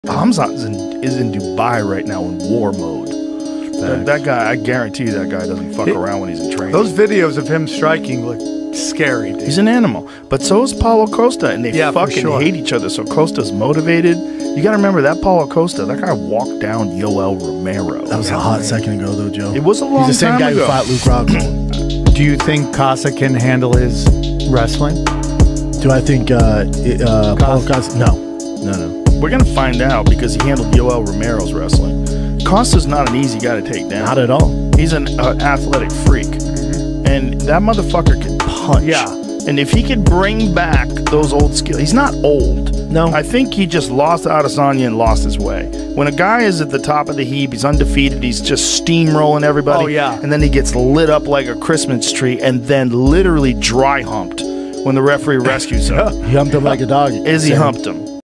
Joe Rogan on Khamzat Chimaev vs Paulo Costa
Joe Rogan and Brendan Schaub discuss the upcoming UFC fight between Khamzat Chimaev and Paulo Costa. Joe Rogan thinks it will be a tough matchup for Paulo Costa since Khamzat wrestling is so elite.